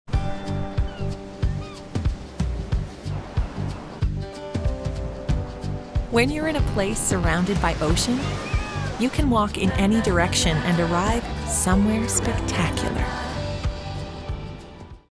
Big Beauty in St. John's, Newfoundland and Labrador.
Where can you step out of your downtown hotel and, a few minutes later, be on a walking trail or tour boat with views of colourful fishing boats, icebergs, and whales? Only in St. John's, Newfoundland and Labrador.